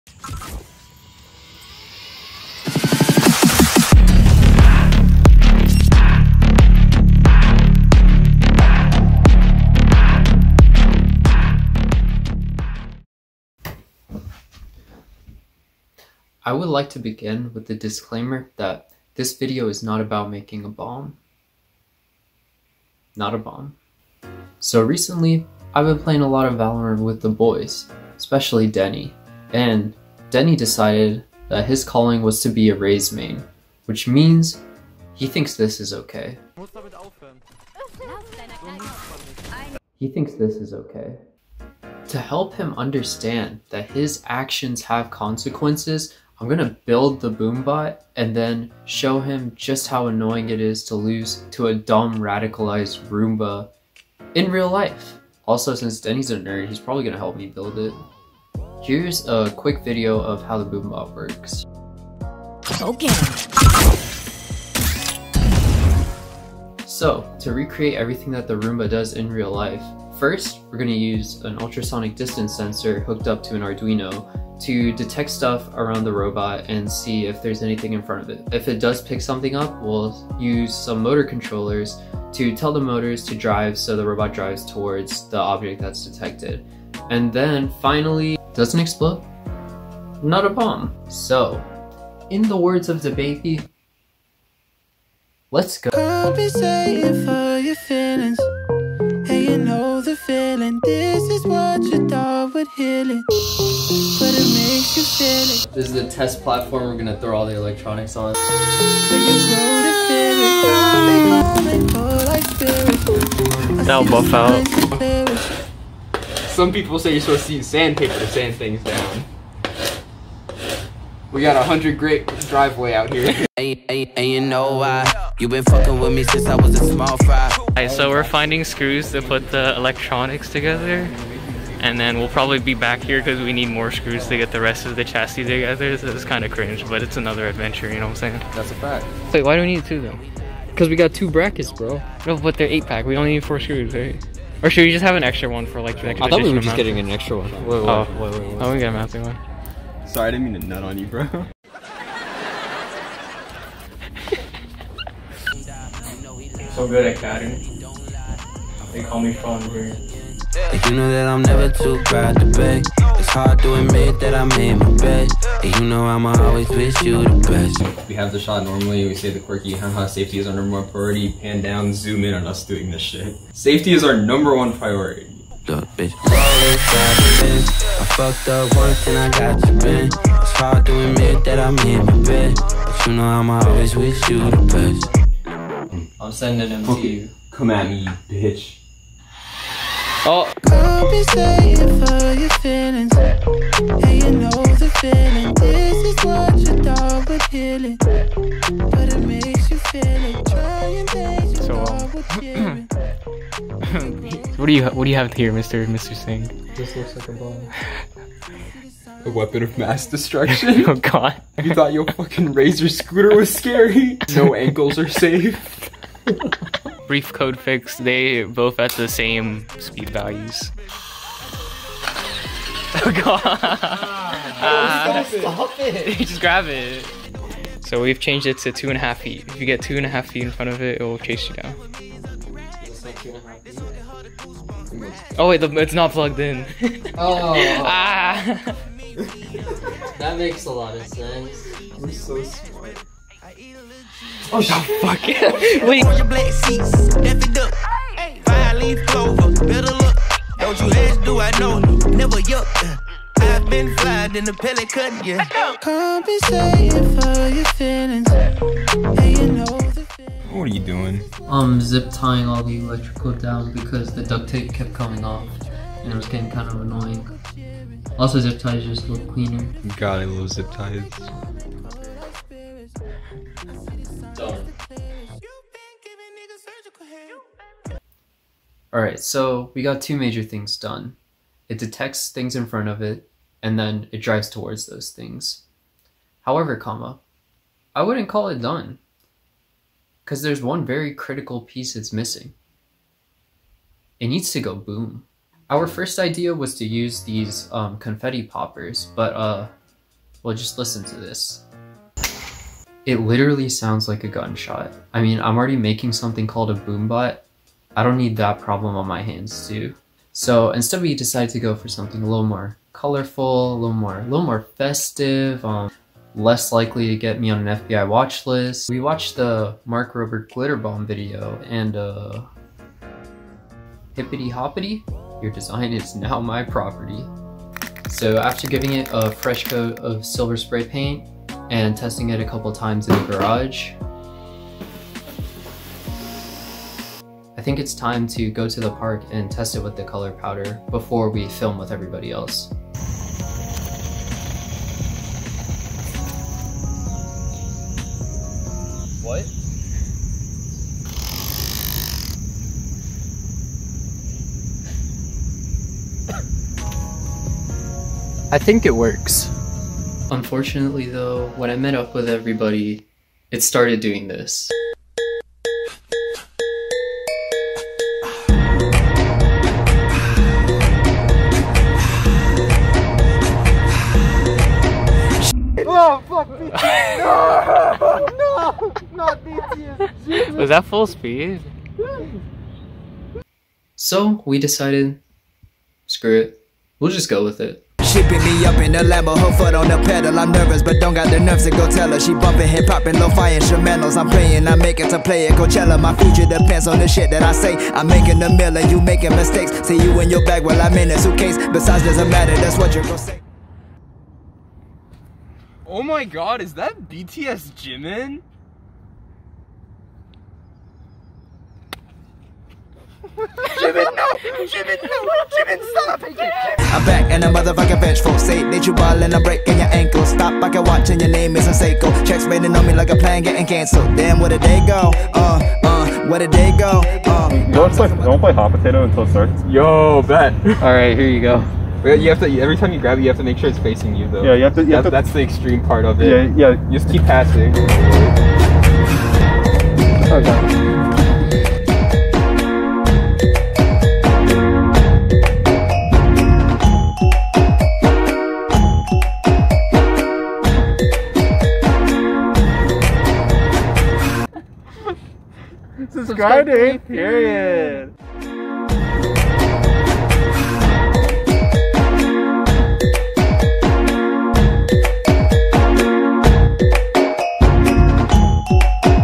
We Built Raze's Exploding Boombot
Making Raze's Boombot (Roomba) from Valorant. It might decide to chase you down, and if it gets that far it should explode with color powder (results may vary).

Uses an Arduino Uno and Distance Sensor to detect people in front of it, then drives towards them and explodes like the Mark Rober glitter bomb.

Huge shout-out to Viraj, Denny, Carson, and Edward who also were a huge part of making this project and filming the video of Raze's Boombot IRL. Also shout out shotgun sage for commenting on this video, all my homies love shotgun sage.

Parts Used:
- Arduino Uno
- H-Bridge (L298N)
- Distance Sensor
- Cheap Chinese Motors
- Battery Pack
- A lot of foam board
- A lot of daisy chained wires